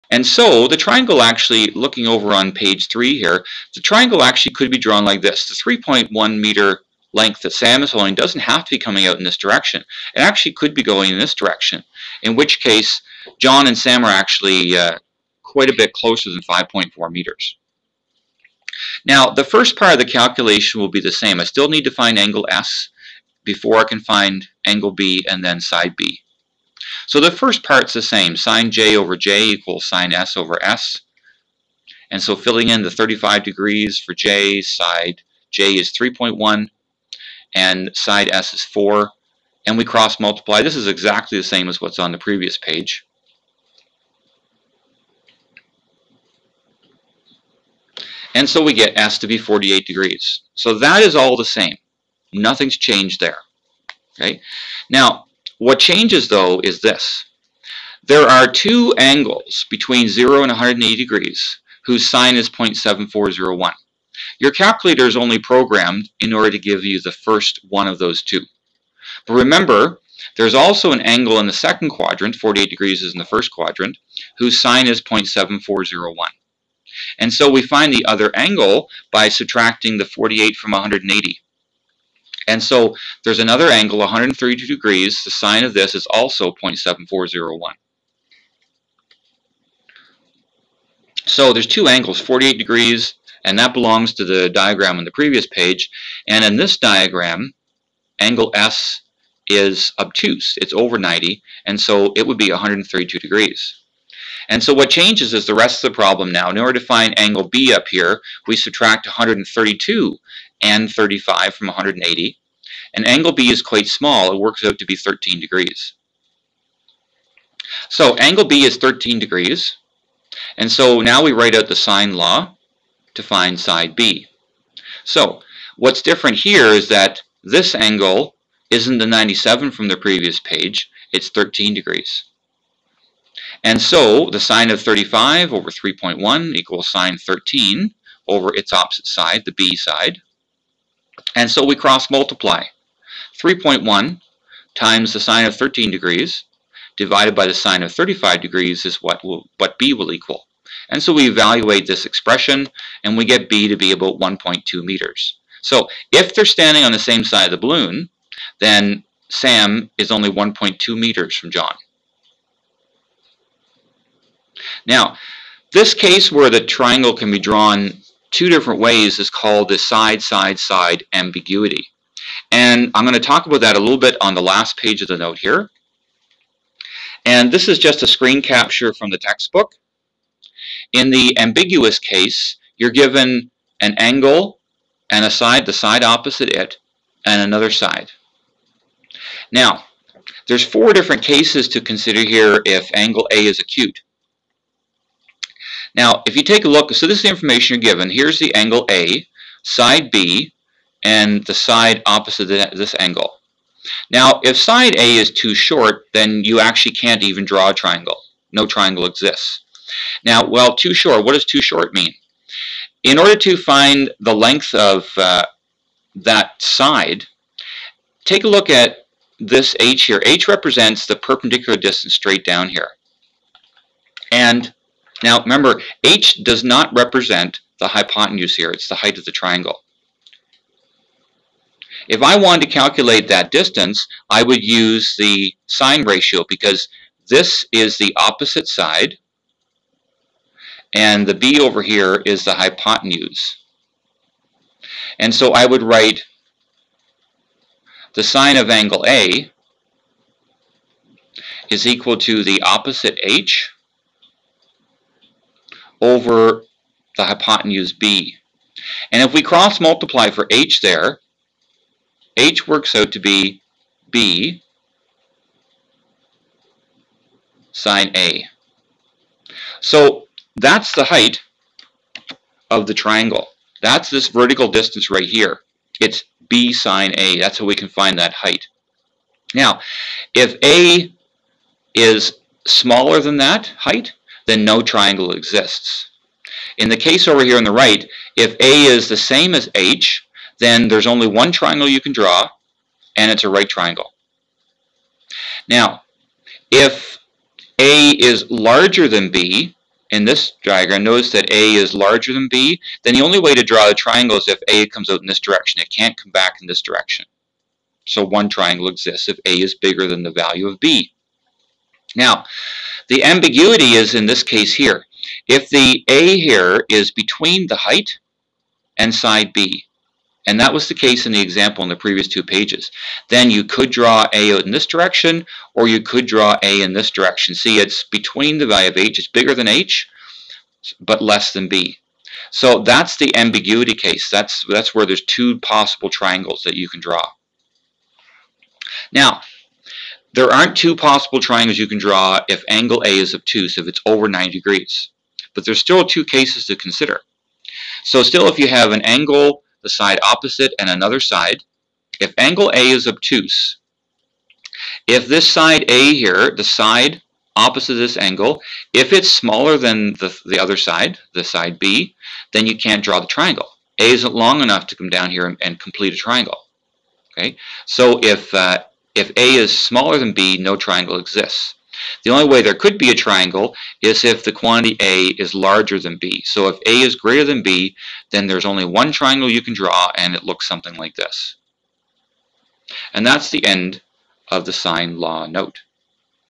The Sine Law Part 2
This lesson shows how to use the Sine law to find angles and sides.  This is the second part of a two part lesson.  This lesson was created for the MCR3U course in the province of Ontario, Canada.